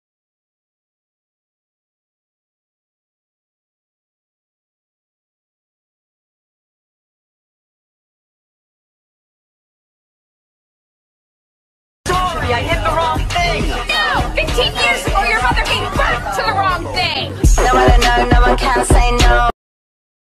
• No, 15 years ago, your mother gave birth to the wrong thing • || gacha meme || OoO Bryrio
Anotherrr fillerrr~ I don't know if I have to repeat this again but, my videos are NOT canon nor going to be very similar in certain ways. Relationships in my AU are mostly fanon and do affect the characters and situations in said AU.
Let's be real here, if canon Bryan (who is supposedly DEAD) saw this situation happening, he probably wouldn't act like what this video shows.

Characters:
Some new camper (Mine)
Bryan ( @thefamousfilms)
Mario ( @MarioMania)

From who I learned the ahoge/eye movement: @Gacha Siri